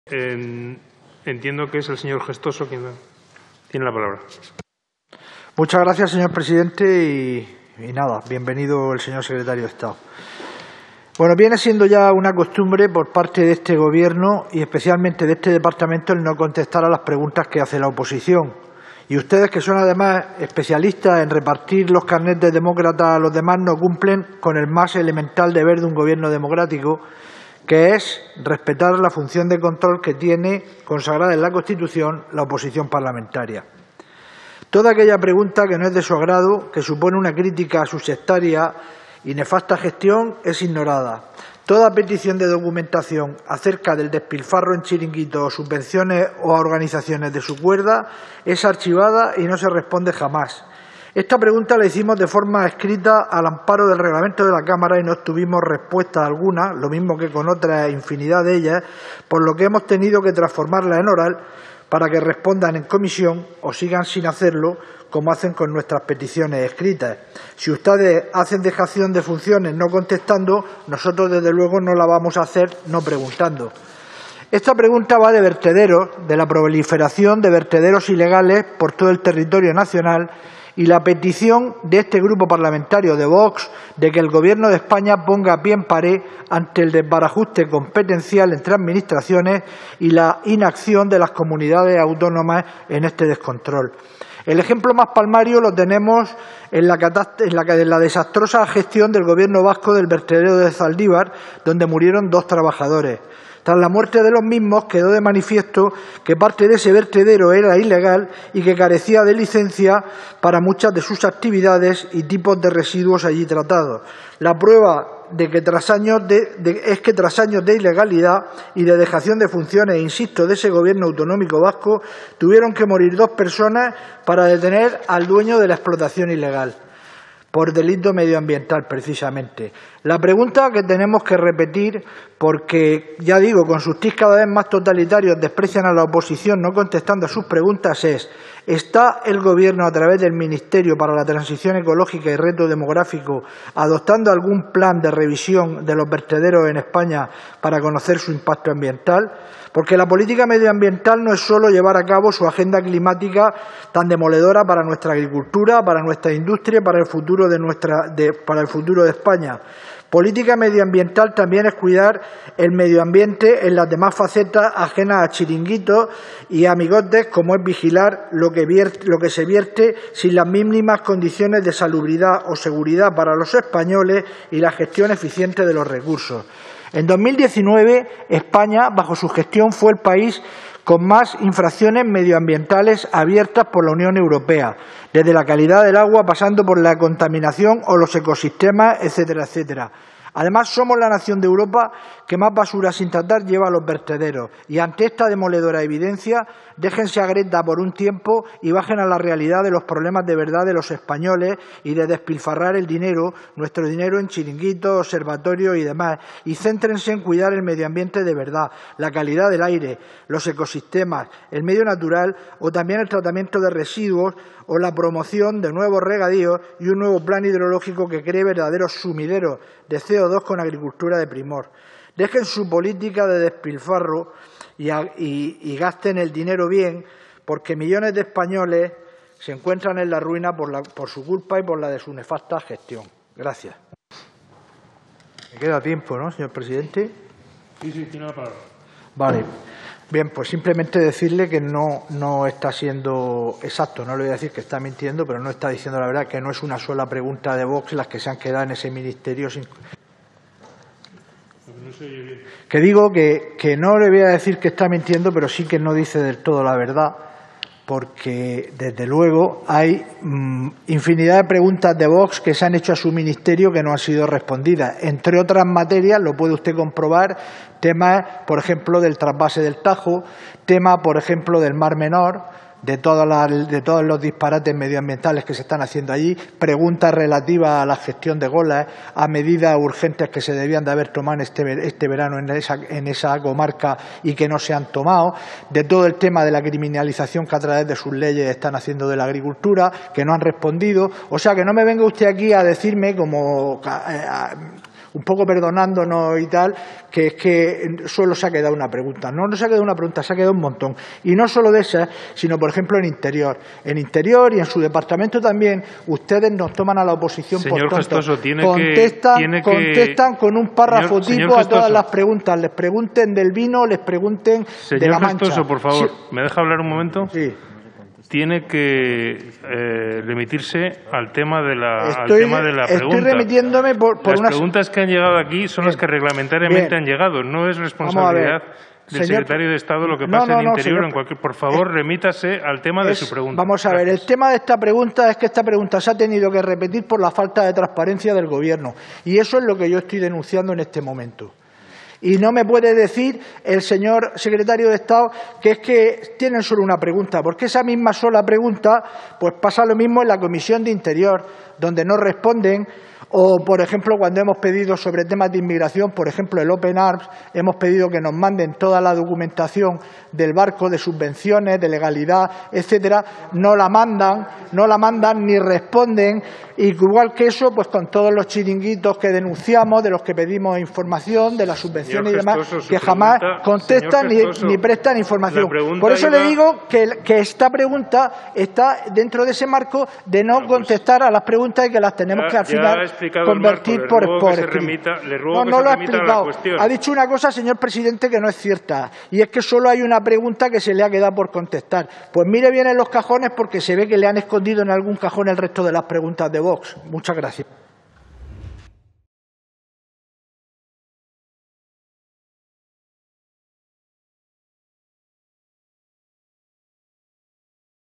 Gestoso, al secretario de medioambiente: 'Su agenda climática es demoledora para la agricultura'
El portavoz de VOX en la Comisión de Transición Ecológica y Reto Demográfico pregunta al Gobierno si está adoptando algún plan de revisión de los vertederos para conocer su impacto ambiental y lamenta que España fuera en 2019 el país con más infracciones medioambientales abiertas por la UE.